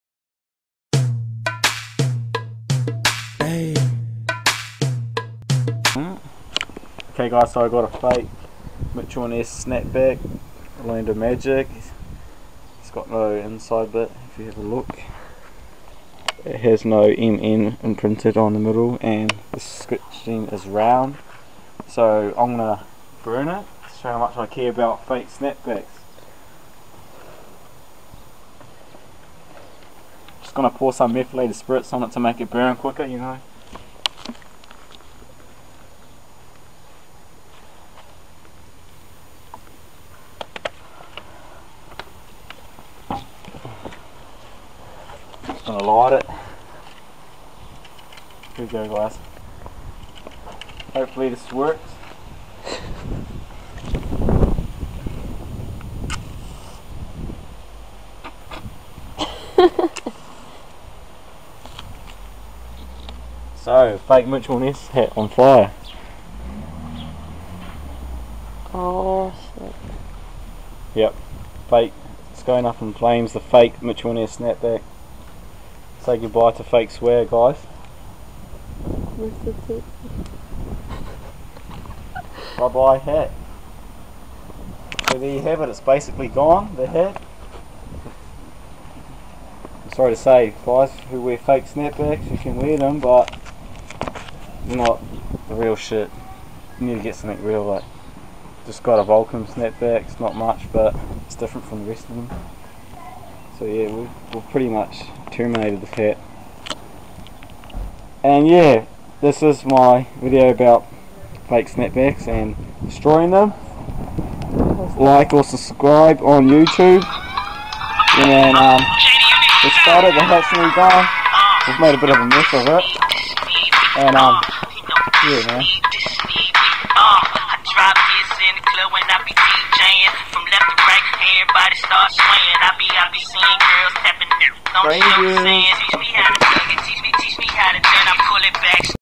Destroying a fake Mitchell & Ness snapback
Burning a fake Mitchell and Ness snapback my noelza style!